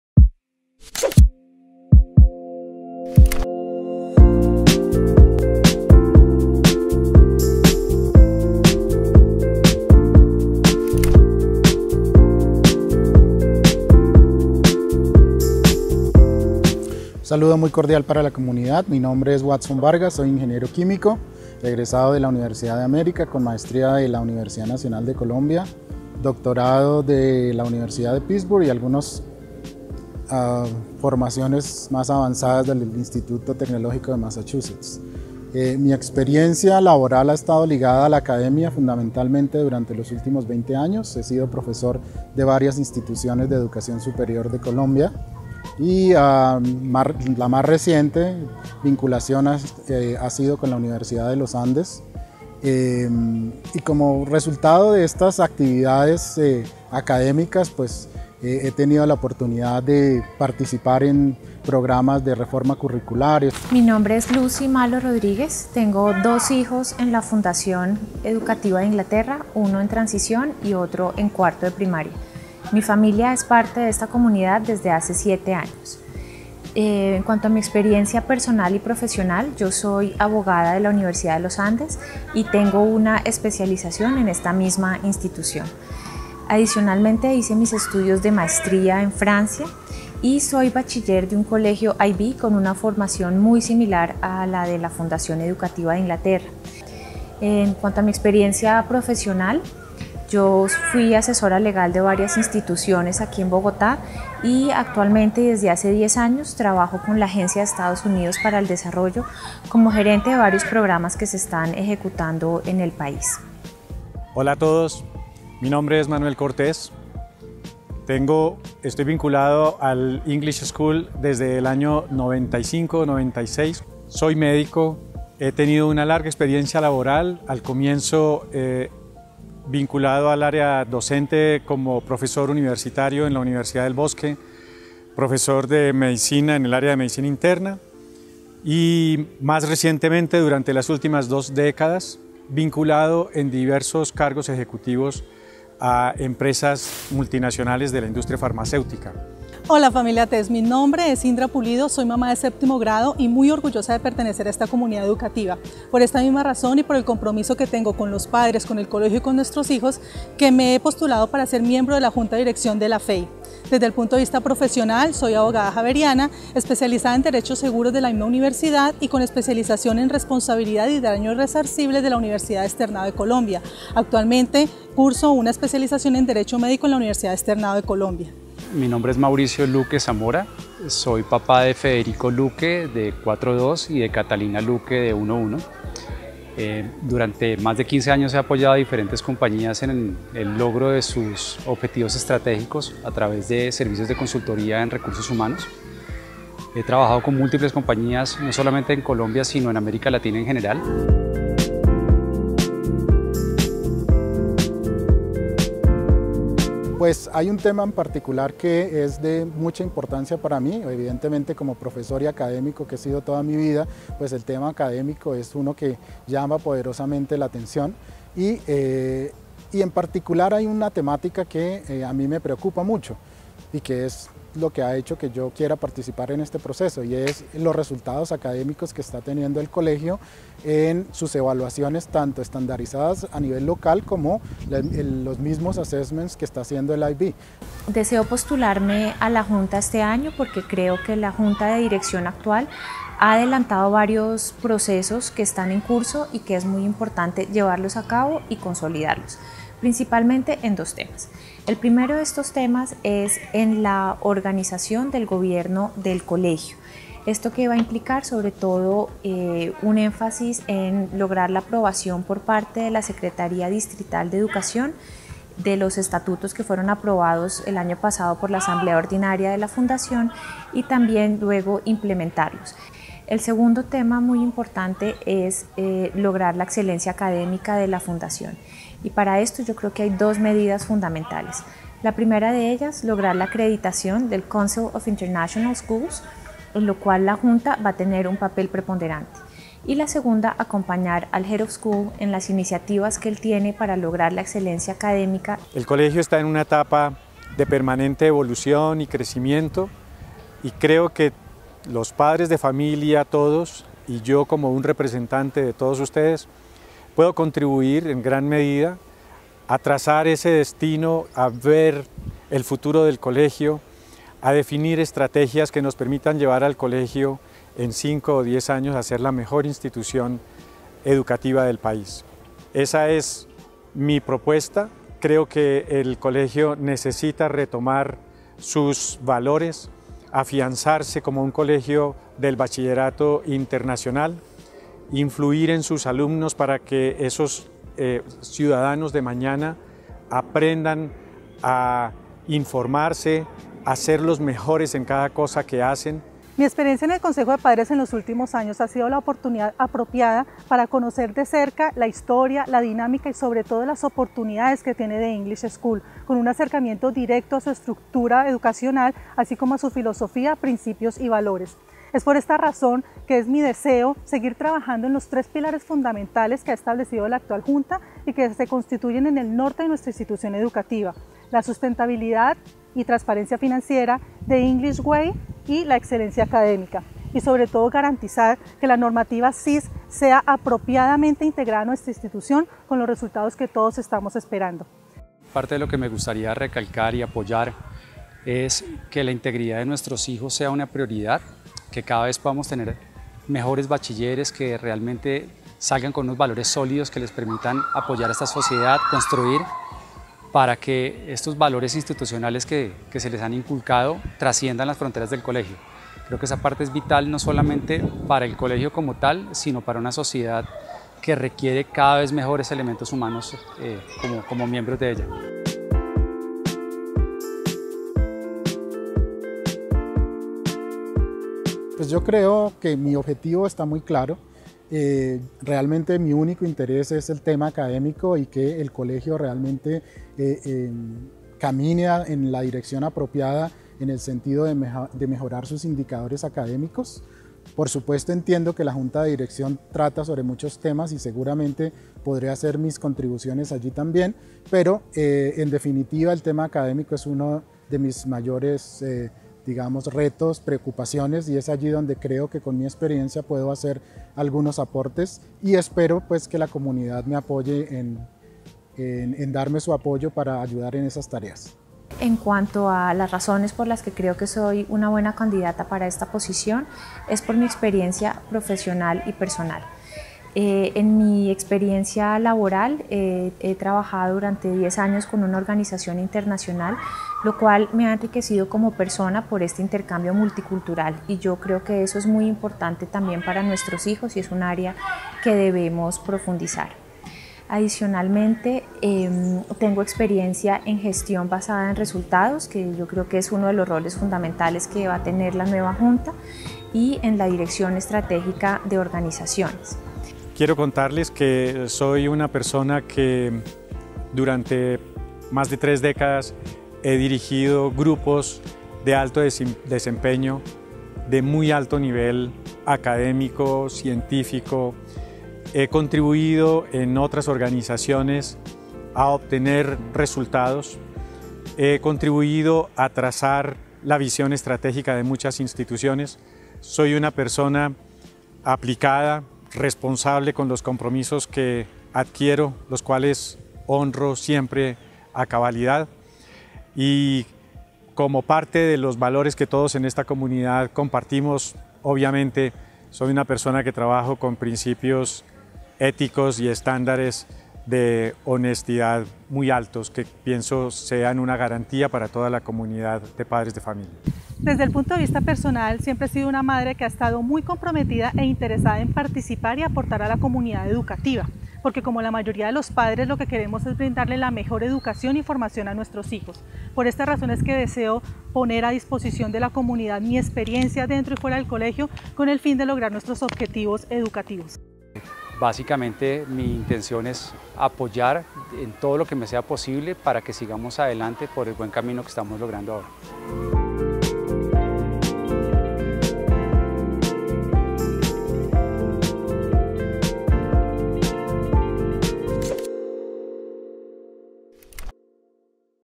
POSTULADOS JUNTA DE DIRECCIÓN
Presentación de postulados para ser miembro de la Junta de Dirección de la Fundación Educativa de Inglaterra.